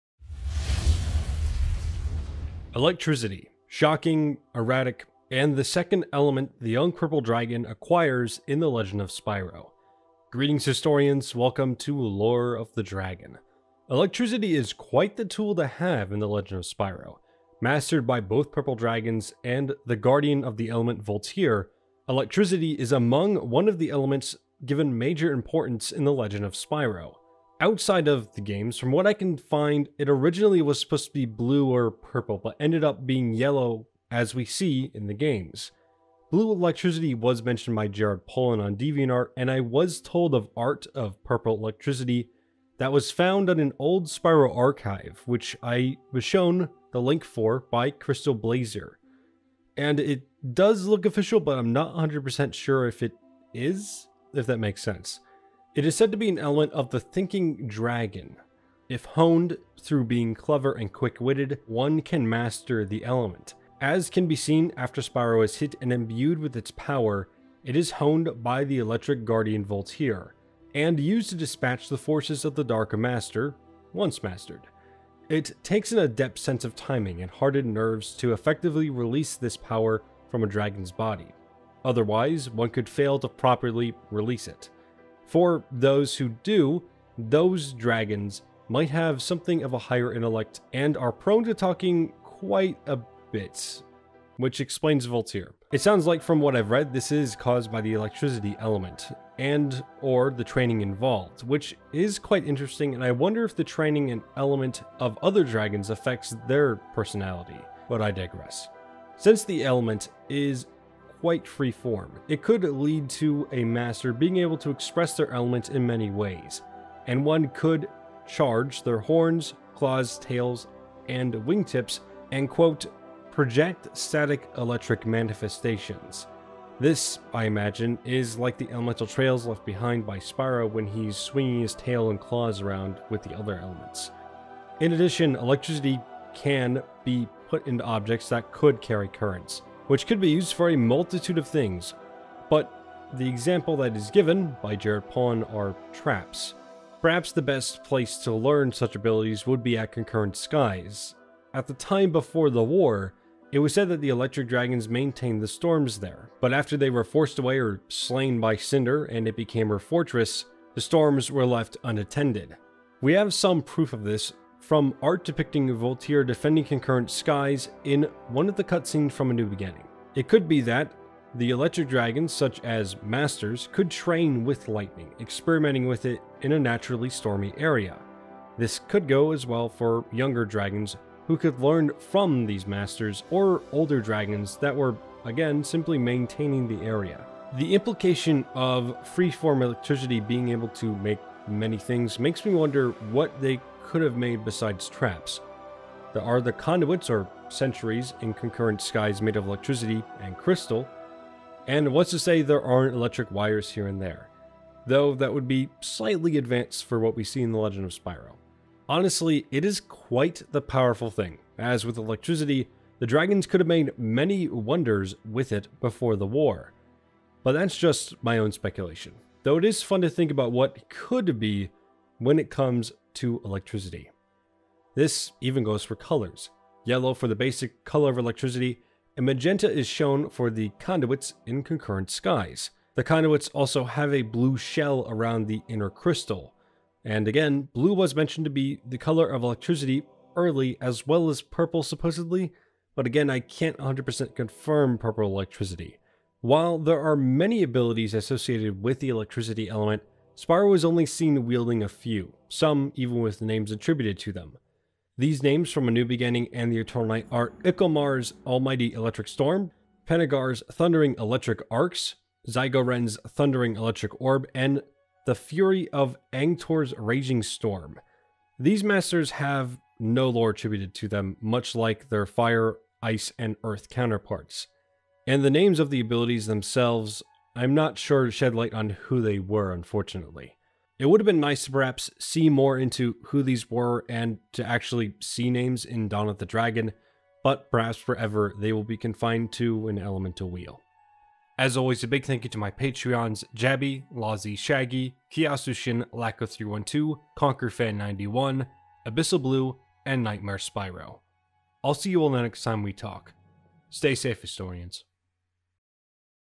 Spyro Lore - The Element of Electricity (The Legend of Spyro Lore)
Greetings, Historians! Today, I'm talking about the second element ever used in the Legend of Spyro: Electricity.